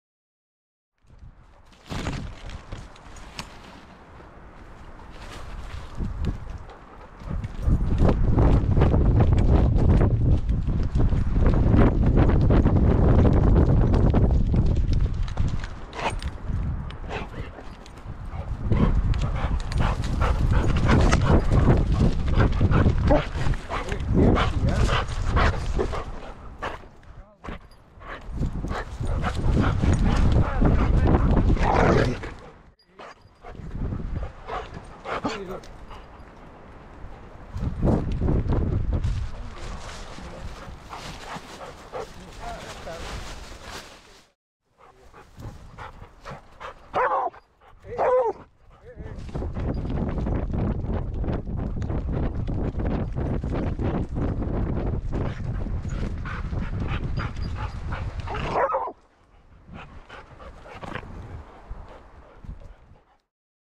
ATO meets a Friend !!!
English Pointer "ATO" meets one of his friends in the forrest of Bandol at the French Riviera.